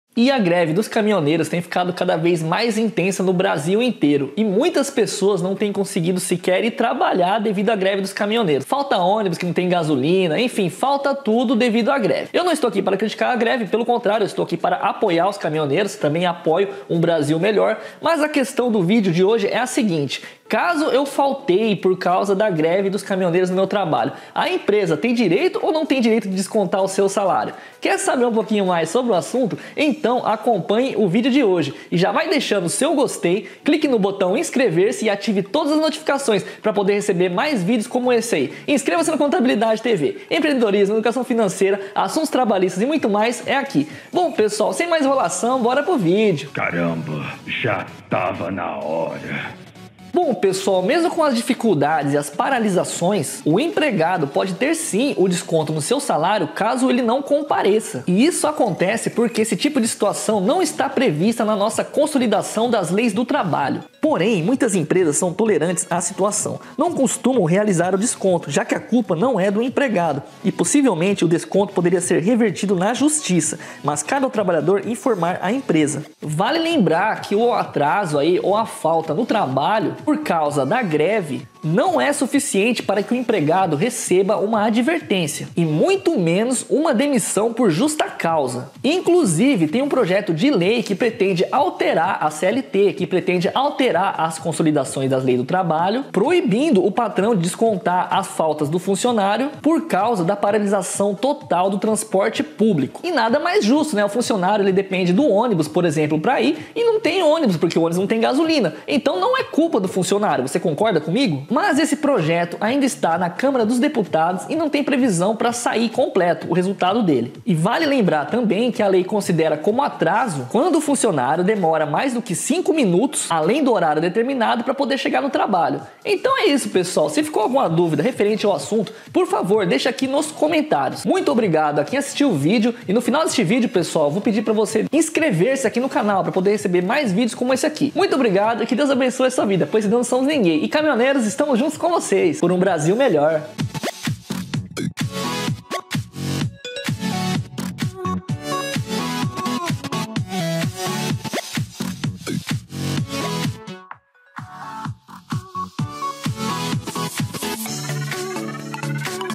🔴 Atrasei no trabalho devido a greve dos caminhoneiros, a empresa pode descontar do salário ?
Caso o trabalhador faltar ou atrasar devido à greve, a empresa pode descontar do salário?